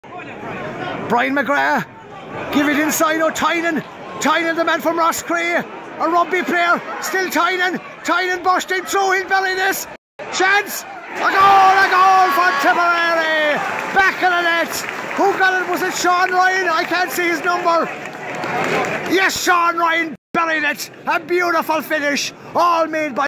Liam Cahill gets first competitive win as Tipperary boss
Liam Cahill gets his first competitive win as Tipperary's senior hurling manager. Tipperary 2-22 Clare 0-20.